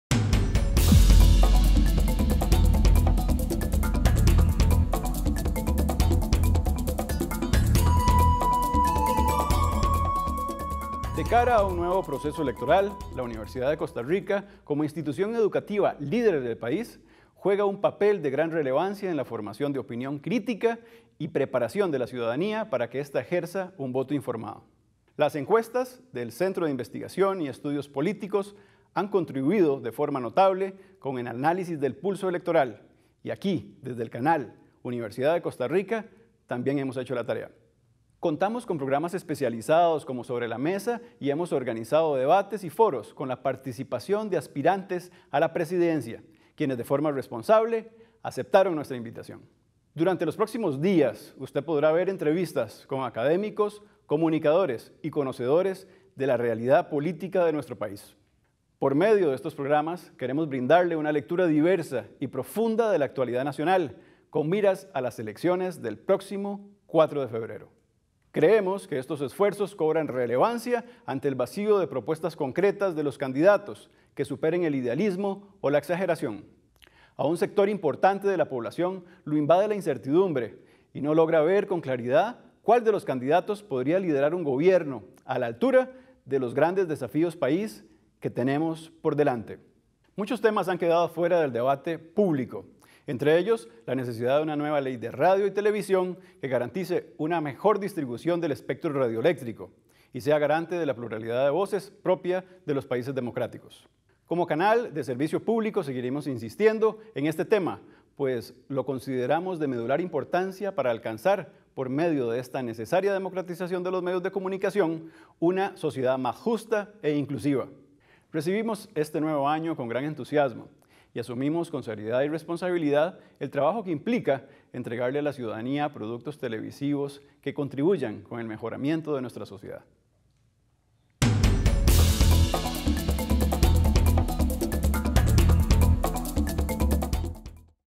Editorial del Director: Elecciones 2018
En CanalUCR hemos estado trabajando para llevarle a nuestro público las herramientas para emitir un voto2018 crítico e informado, por medio de la realización de producciones afines al quehacer electoral, las cuales se estarán viendo en nuestra pantalla y redes sociales.